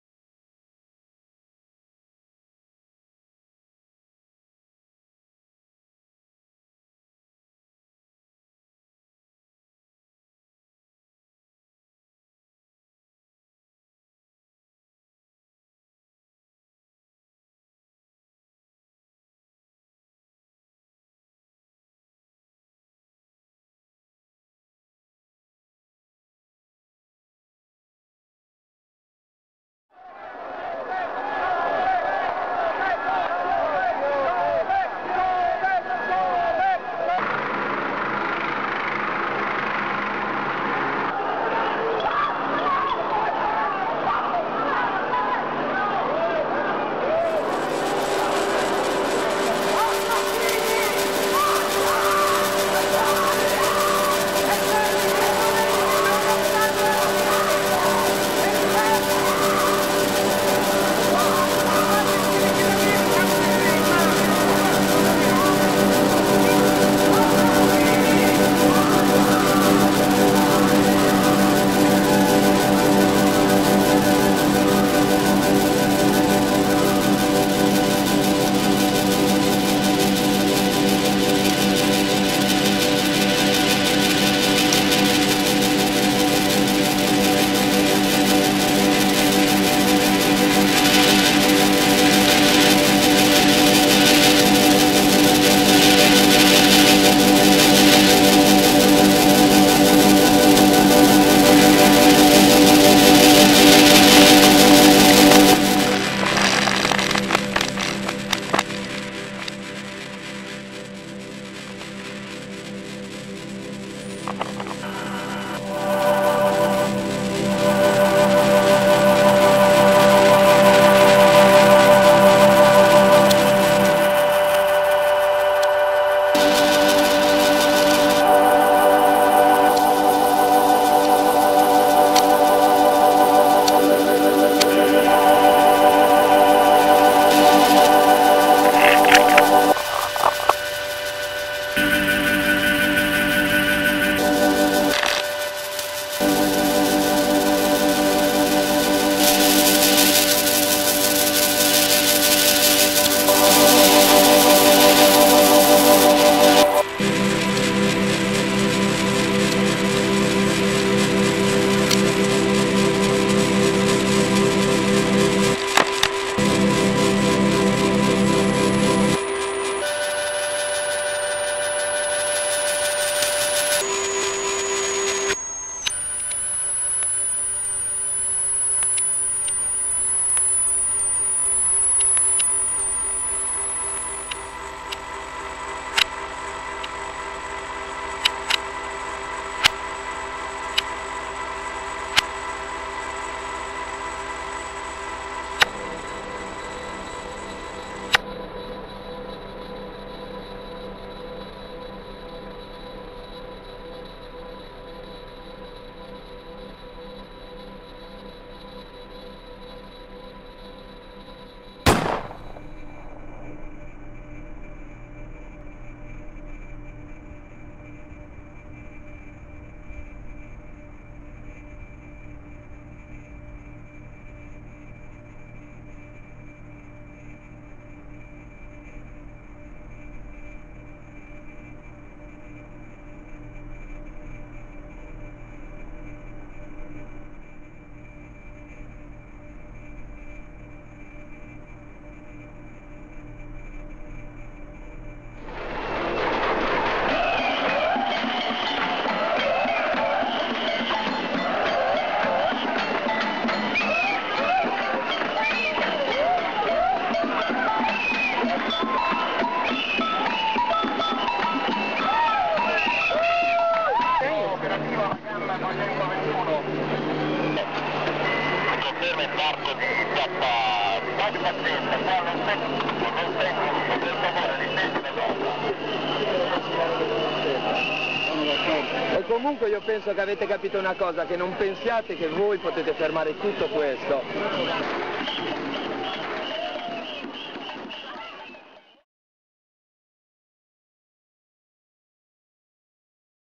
BLUE-UP G8, (im)morta(lata) visione
Video "sperimentale" basato sulla (de)costruzione digitale dell'immagine video, (de)costruzione (de)strutturata dalla sinergia tra le immagini primordiali (specificatamente al formato elettronico-digitale) e la musica elettronica/sperimentale.
Il tema del video è la violenza delle forze dell'ordine durante il G8 di Genova nel 2001 e sull'indifferenza della nostra classe politica nel far luce su quei giorni oscuri. Luce che viene data formalmente nel video attraverso la riduzione delle immagini di quei giorni a luce (propria del mezzo)/colore/linee per sottolineare tramite l'essenza profonda del linguaggio video-digitale la morta visione di quei giorni e per riflettere sulla dinamica stessa della visione.
Il titolo vuole essere un richiamo esplicito a "BLOW-UP" di Michelangelo Antonioni.
Quel giorno tutti(?) hanno visto!

Marco Di Battista